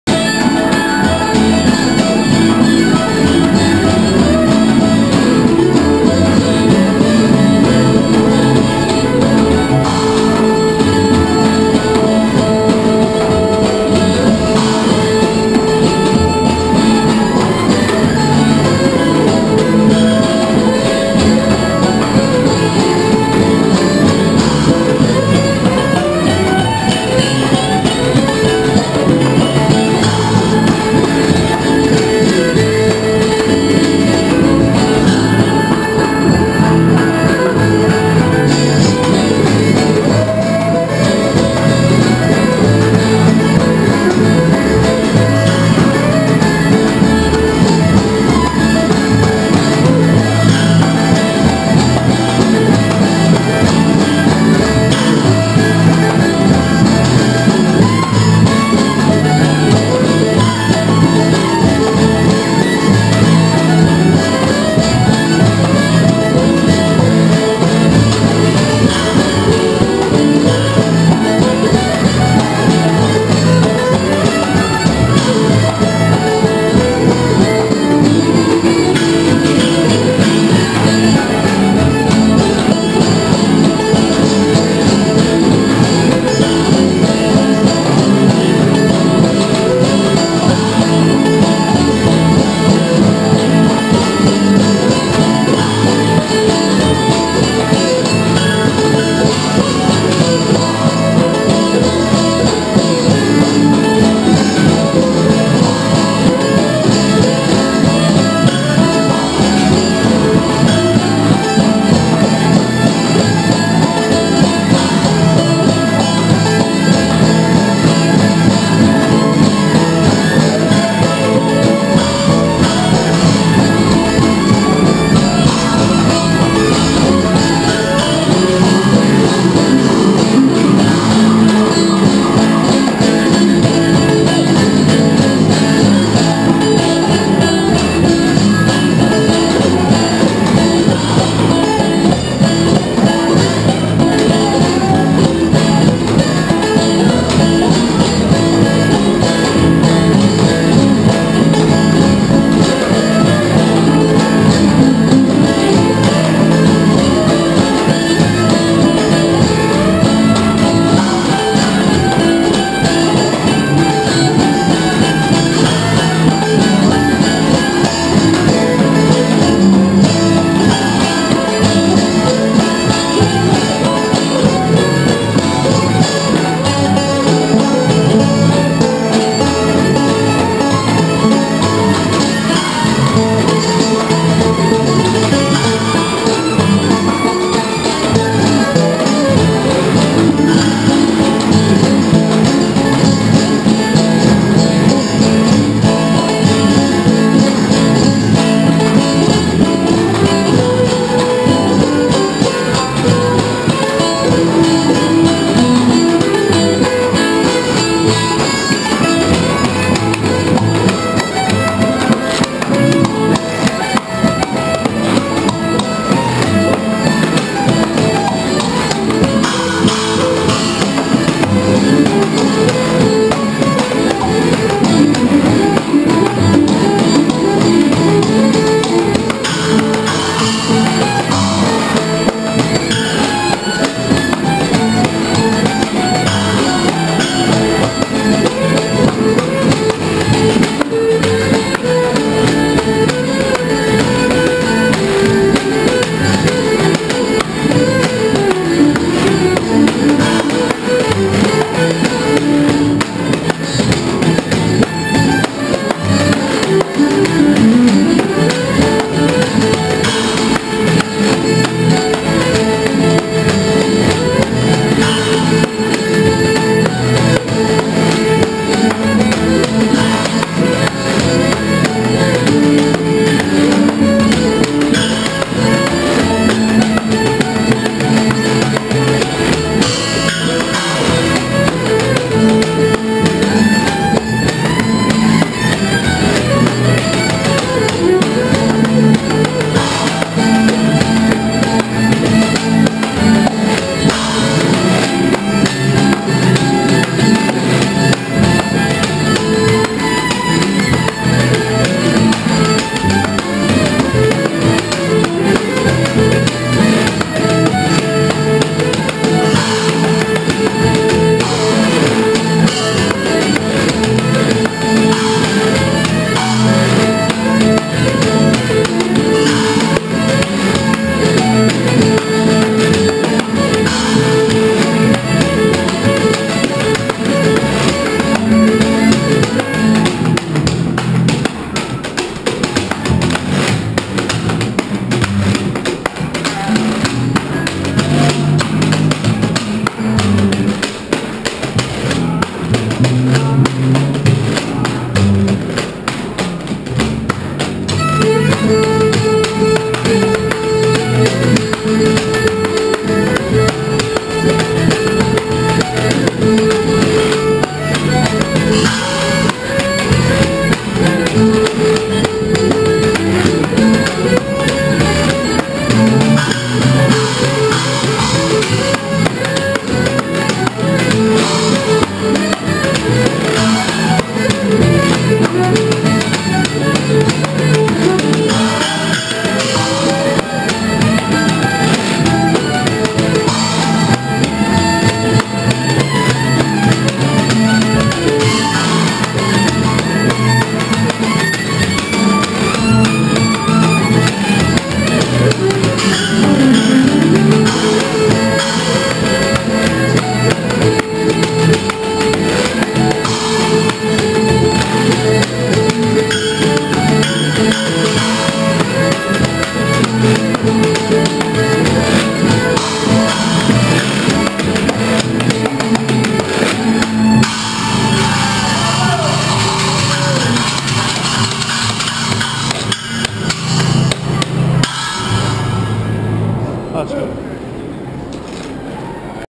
NYC Ebony Hillbillies Live in the New York City Subway
I stumbled upon this amazing band at the 42nd St/Time Square station. They are playing on Tuesday, Jan 24 at 10:PM at the McKittrick Hotel in New York. ( 532 West 27th St. Between 10th & 11th Aves.)